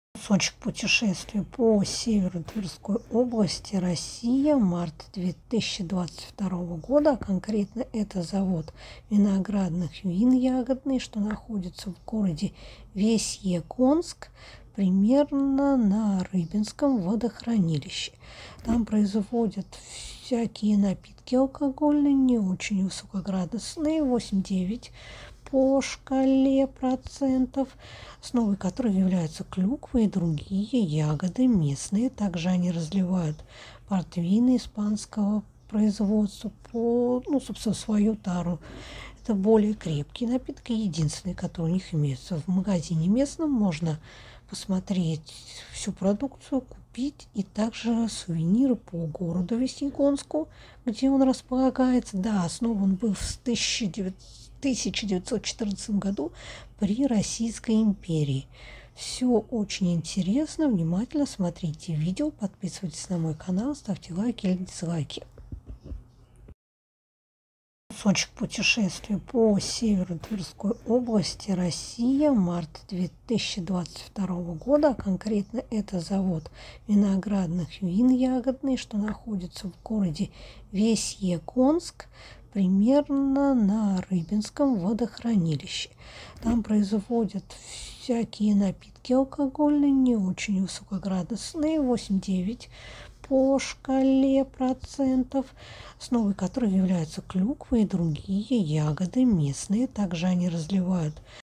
Vesyegonsky zavod wine review Russia March 2022/весьегонский завод винный обзор Россия марта 2022
Просто часть путешествия, когда вы оказываетесь на современном заводе ягодных вин. Прекрасен в плане осмотра производства и покупки сувениров.
Дегустацию, как и полноценную экскурсию вам проведут точно. Классное мероприятие и приятные цены.

Just part of the journey when you find yourself in a modern berry winery. Excellent in terms of viewing production and buying souvenirs. Tasting, as well as a full-fledged tour, will be held for sure. Great event and great prices.